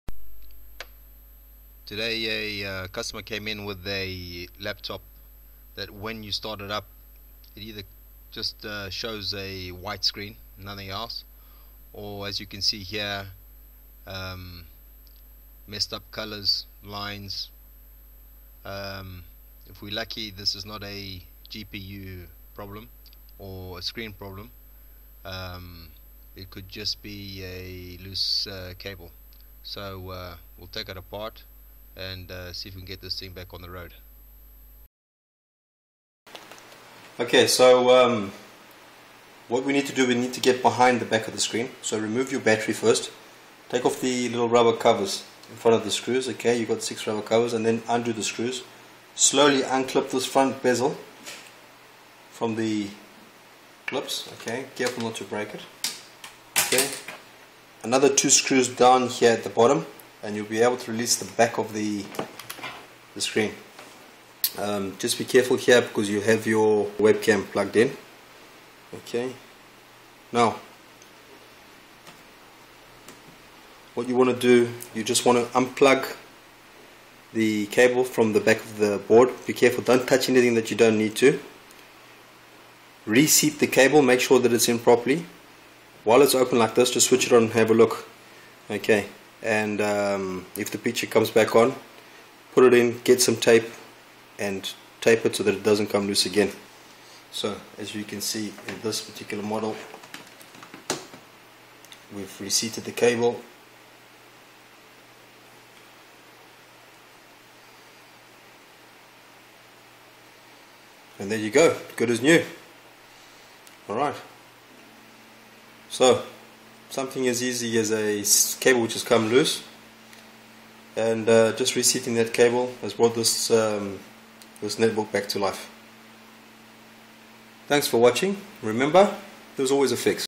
Laptop white screen repair acer
In this video I troubleshoot and repair this acer laptop that shows only a white screen or messed up colors.